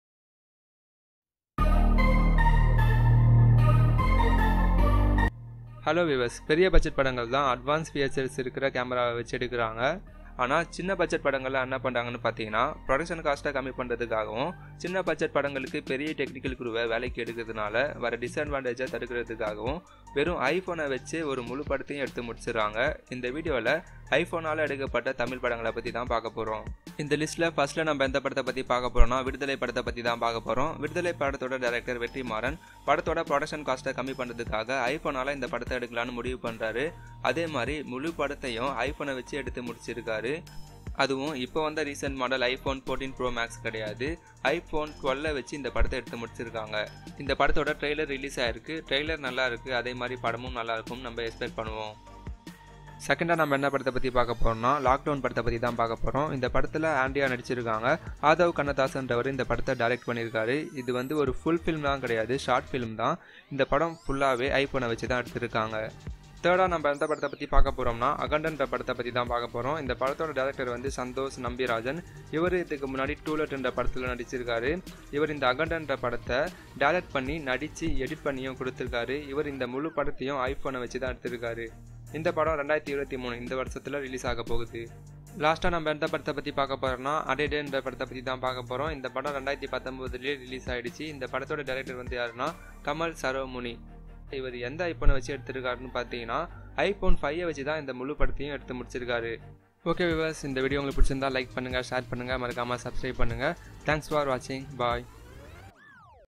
Iphone-ல் எடுக்கப்பட்ட தமிழ் படங்கள்|விடுதலை|Tamil films that were shoot by iphone|viduthalai
This video is about the films which were shoot by Iphone.
Viduthalai, promoted as Viduthalai Section 1, is a forthcoming Tamil-language period wrongdoing spine chiller movie composed and coordinated by Vetrimaaran. Delivered by Elred Kumar under the standards of RS Infotainment. The film stars Soori and Vijay Sethupathi. This film was introduced by RS Infotainment.
The trailer of Vetrimaaran's Viduthalai Section 1 was uncovered on Wednesday.

A period wrongdoing show, the film focuses on a police activity to cut down 'Individuals' Military', an imaginary dissenter bunch that has waged war and is concealing in a woods. It stars well known entertainer Soori in his most memorable job as a legend. Vijay Sethupathi is having a significant impact, as the head of the dissidents, in a lengthy appearance. The cast additionally incorporates entertainer chief Gautham Vasudev Menon.
Set to deliver in two sections, Viduthalai is adjusted from a brief tale Thunaivan composed by Jeyamohan.

The trailer of Section 1 exhibits the hardships of a constable, Kumaresan (Soori), who is only a machine gear-piece in a gigantic framework that is on a mission to get Perumal Vaathiyaar (Vijay Sethupathi). The subtle head of Individuals' Military, Perumal, has pursued a conflict against the experts for bothering and tormenting the ladies of his town.
The whole film has been shot in an iPhone, which is utilized to control creation costs. The shooting of the film occurred in Sathyamangalam for quite a long time, and the group moved to Chengalpattu in April 2021, in which the group raised a gigantic police camp set, and shooting of the film occurred for 20 days.